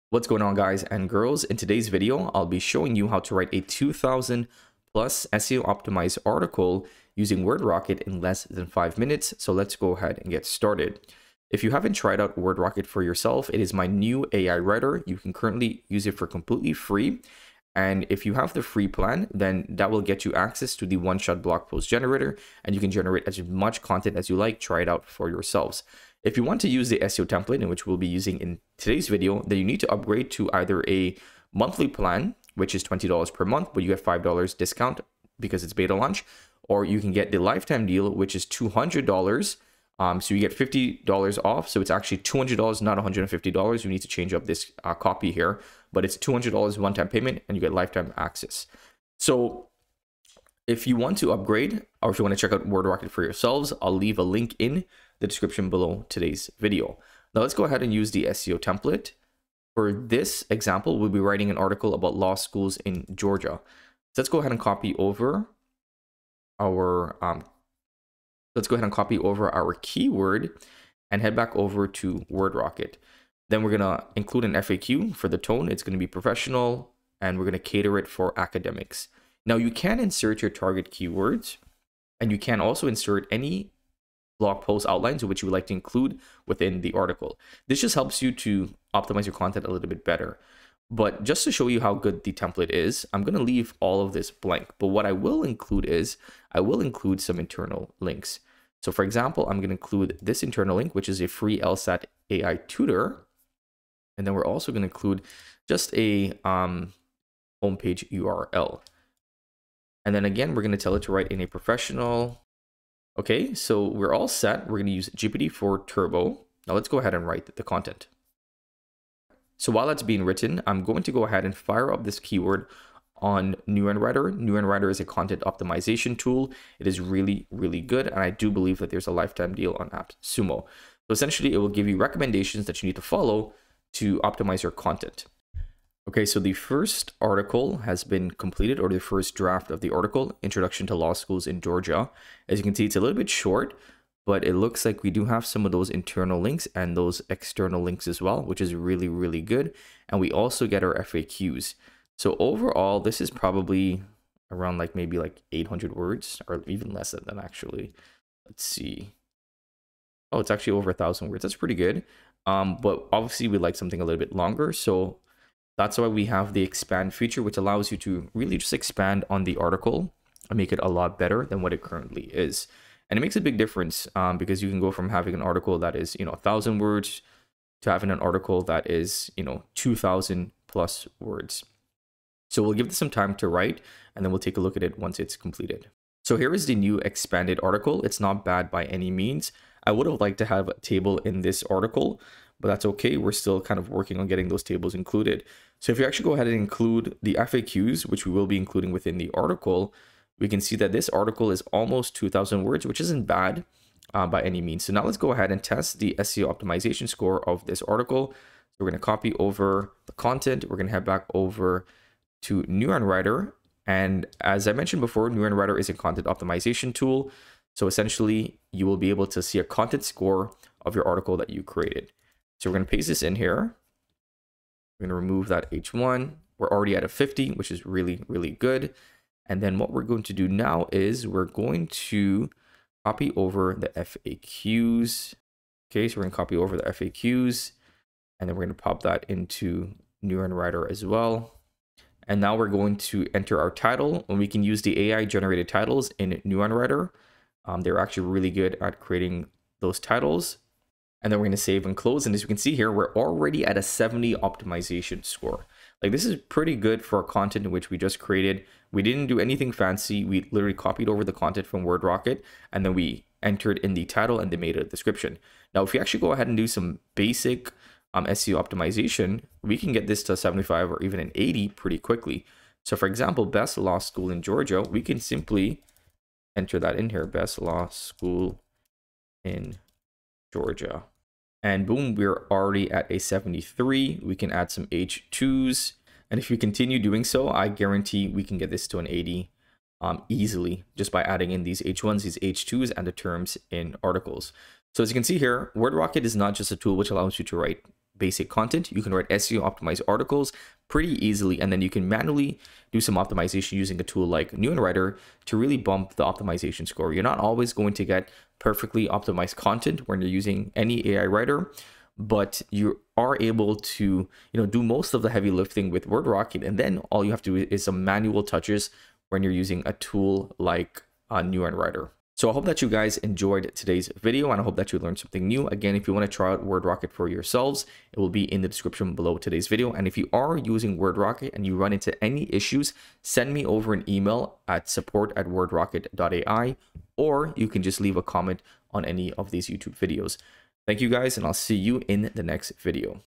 How I Write An SEO Optimized Blog Post In 5 Minutes (75+ Optimization Score)
Discover the future of SEO-optimized content creation with our latest video tutorial! Dive into the revolutionary process of crafting high-ranking blog posts in just 5 minutes using WordRocket AI.
$5 Off Monthly Plan Code: Betalaunch
$50 Off Lifetime Deal Code: 50OFF

This game-changing AI writer tool transforms your topic ideas into fully optimized articles effortlessly. Say goodbye to hours of writing and hello to instant, quality content that scores 75+ on NeuronWriter's optimization scale.

In this video, we'll take you step-by-step through the entire process: from inputting your initial topic to witnessing the magic of WordRocket AI in action. Watch as we generate a complete, SEO-friendly article in real-time. But that's not all—we'll also guide you through analyzing the content's optimization score using NeuronWriter, proving that quality and speed can go hand in hand.

Whether you're a blogger, content creator, or digital marketer looking to stay ahead in the SEO game, this video is packed with insights, tips, and a demonstration of cutting-edge tools that will elevate your content strategy. Join us to unlock the potential of AI in content creation and achieve unparalleled SEO success.